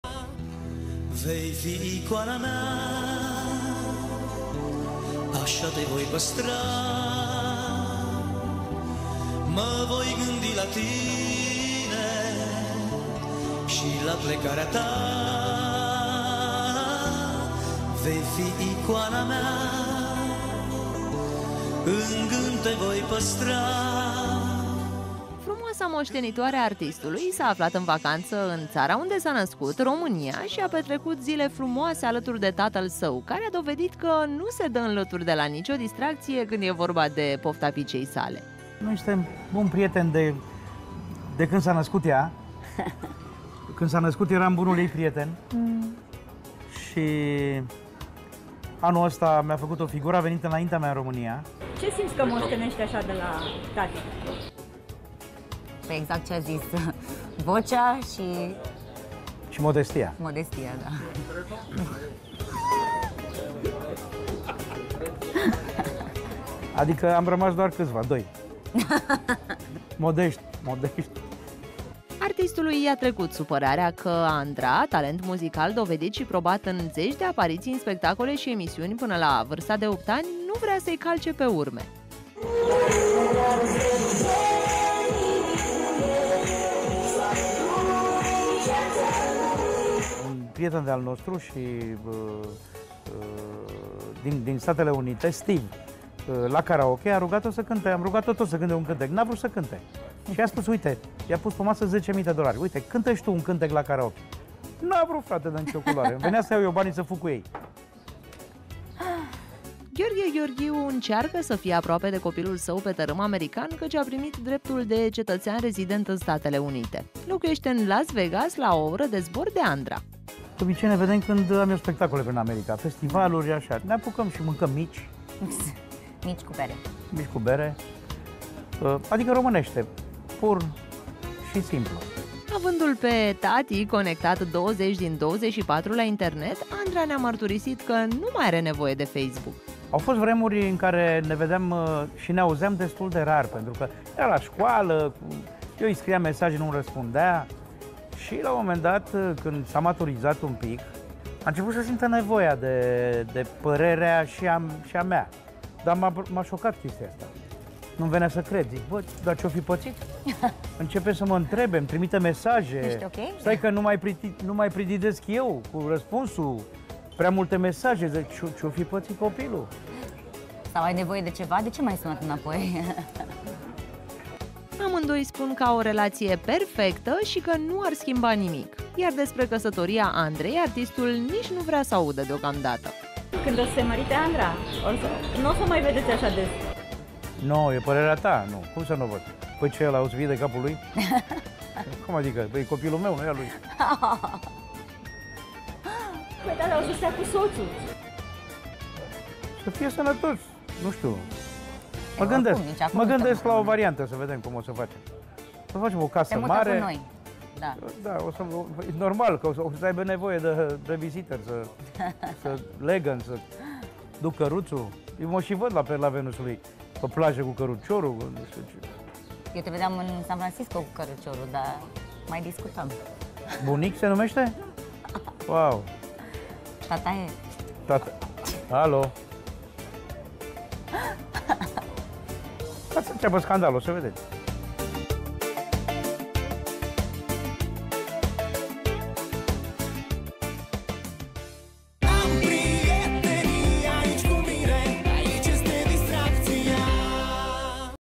Gheorghe Gheorghiu și fiica lui, la plimbare prin centrul Capitalei: ”Moștenesc de la tata vocea”
Frumoasa fiică a lui Gheorghe Gheorghiu s-a aflat în vacanță în România și a petrecut zile frumoase alături de tatăl său.